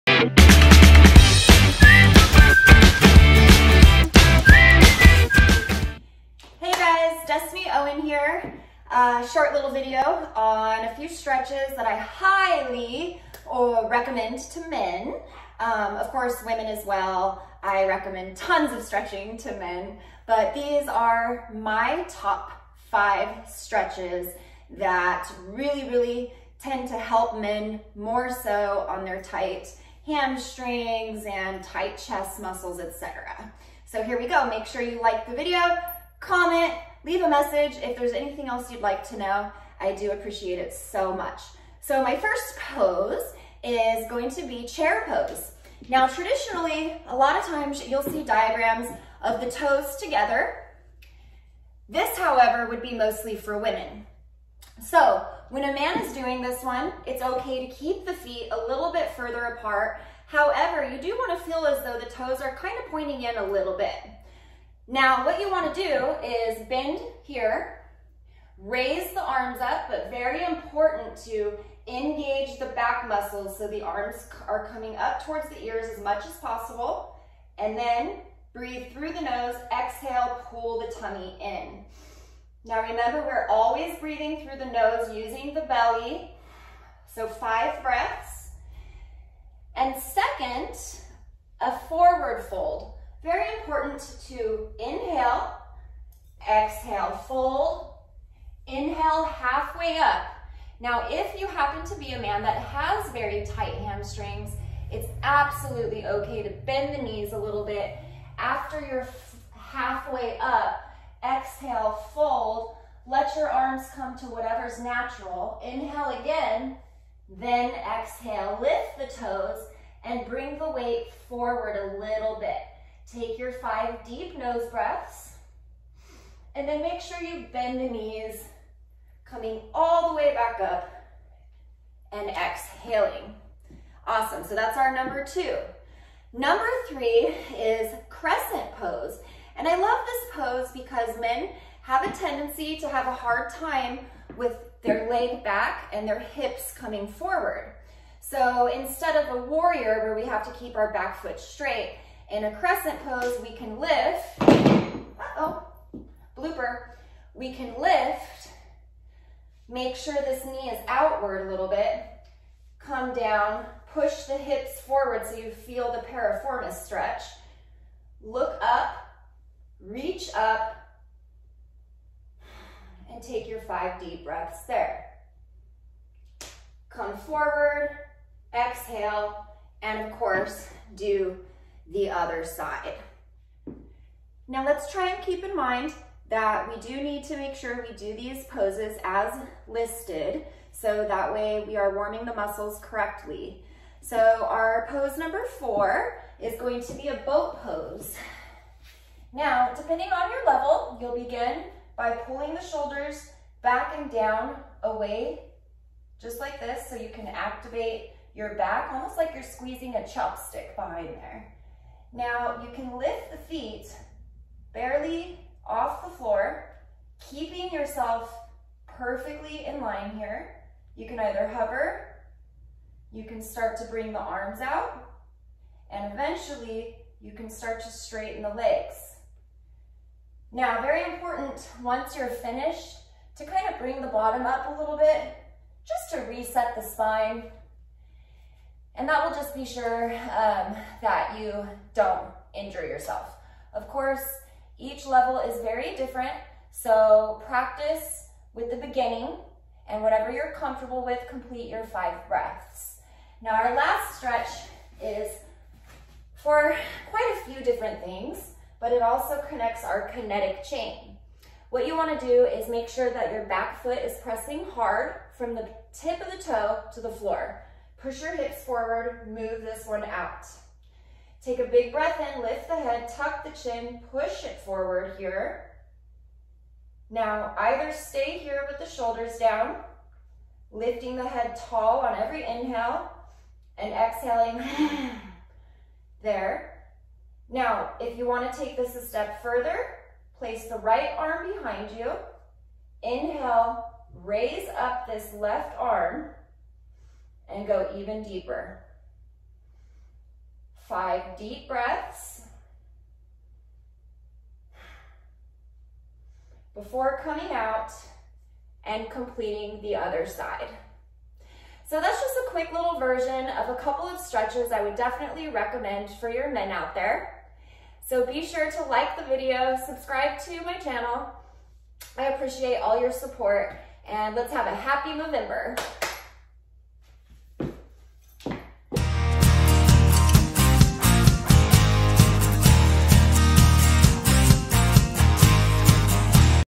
Best stretches for men! Top 5 yoga poses for men with tight hamstrings. stretch quads & tight chest
Professional exercise therapist gives tutorial of Best Stretches for Men! Yoga teacher gives Most recommended yoga poses for men. Stretch quads and fix tight chest problems with chest stretches.

In light of Movember, Destiny Owen gives free health advice for hamstring stretches, lower back stretches, stretches for tight chest, how to fix tight hamstrings. Stretching to increase muscle. Stretches for tight chest in men

For more info please contact anytime:)